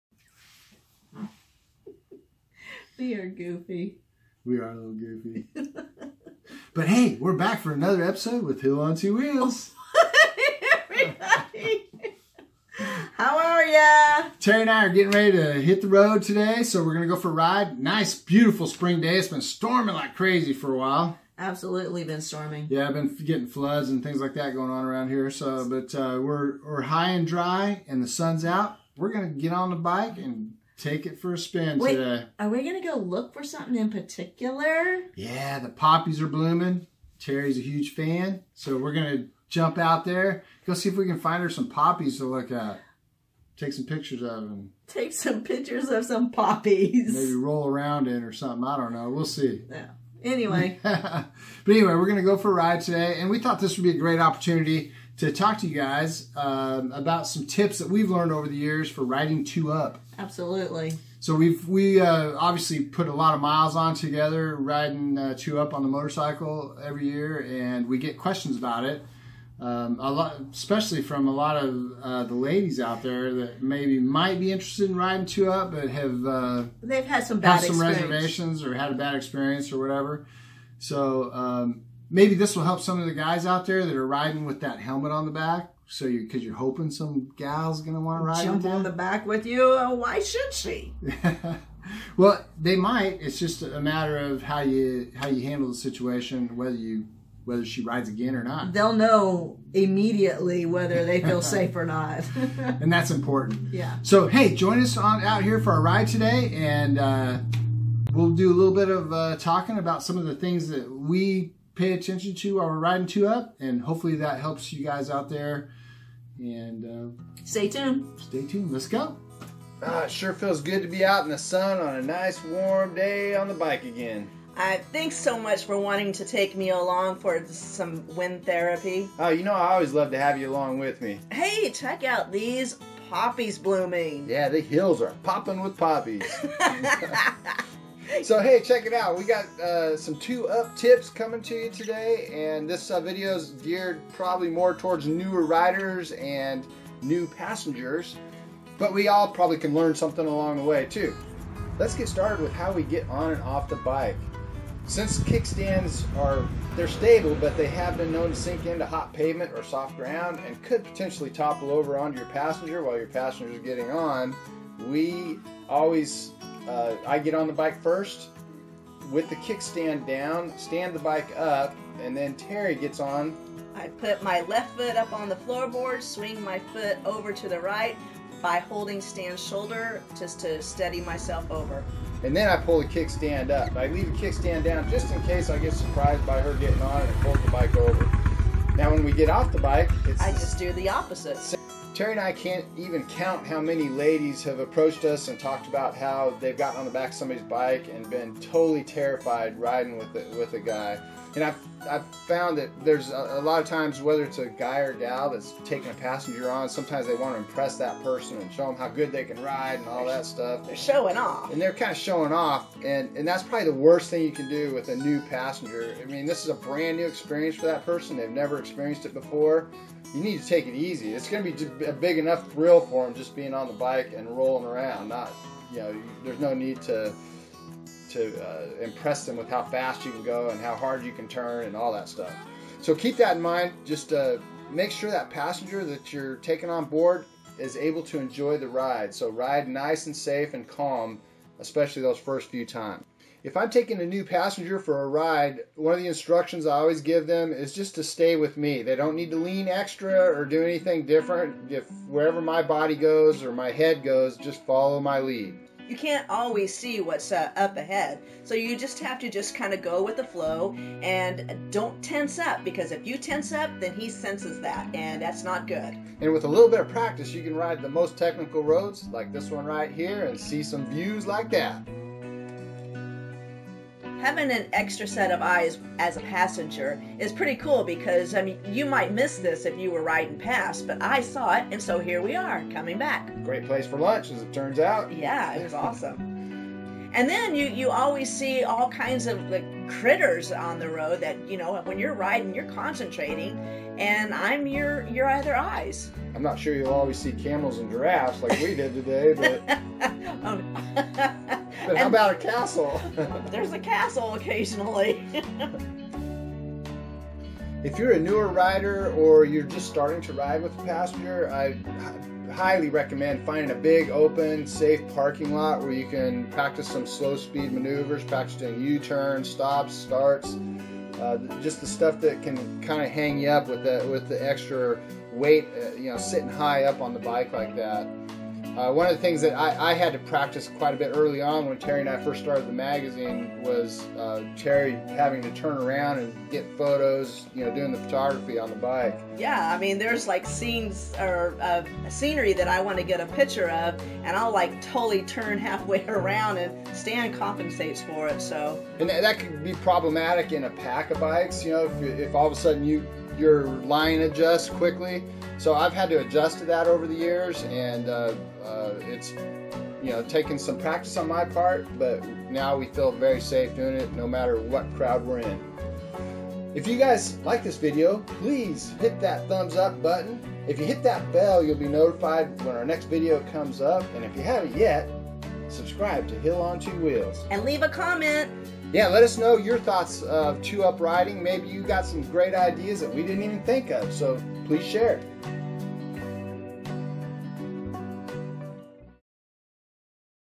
2 UP Riding TIPS
HILLON2WHEELS: Vlog 21.2018   Owner/editors of Thunder Roads Magazine NorCal Stan and Terri Hill (aka Hillon2wheels) 🛣  share their motorcycle riding tips as a couple.